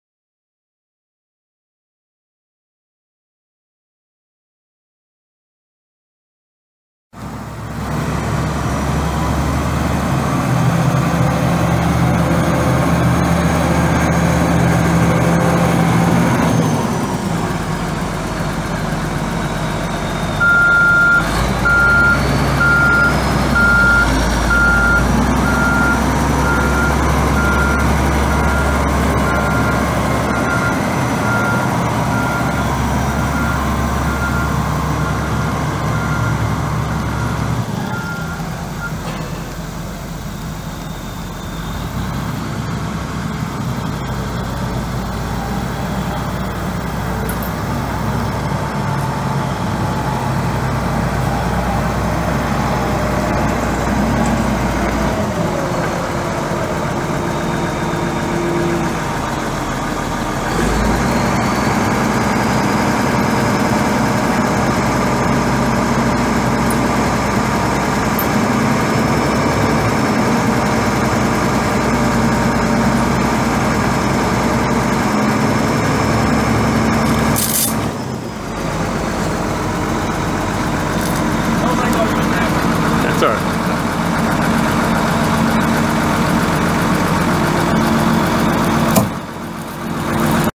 1994 Champion 720A motor grader for sale | no-reserve Internet auction August 31, 2017
1994 Champion 720A motor grader
11,318 hours on meter
Cummins 6CT8.3 8.3L six cylinder turbo diesel engine
201 HP
Serial 45064092
Powershift transmission
8F-8R gears
Differential lock
EROPS
Heat
Suspension seat
Block heater
Front and rear work lights
Tip control
Side shift
Circle slip clutch
Front hydraulic remotes
Michelin 14.00R24 tires
No moldboard
Hydraulic leak
Serial X024489X
Unit# 31
It's located in Saint Joseph, MO, and we're selling it to the highest bidder regardless of price.